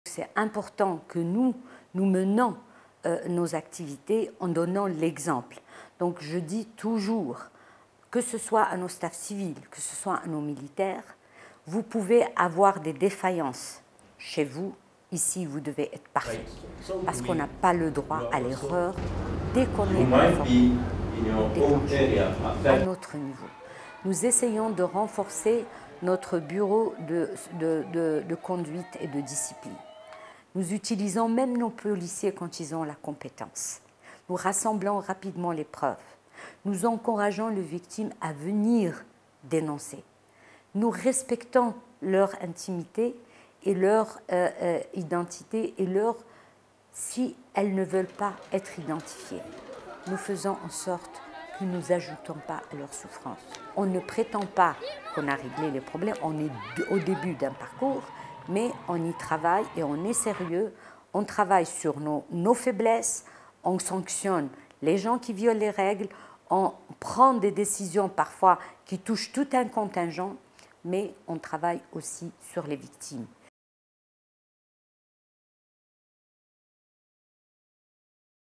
MONUSCO Chief stands up against sexual exploitation and abuse
Since her appointment as the head of our Peacekeeping Mission in the Democratic Republic of the Congo, MONUSCO, in December 2017, Leila Zerrougui has put ending sexual exploitation and abuse by United Nations personnel at the top of her agenda, building on the Mission’s ongoing efforts to solve this problem.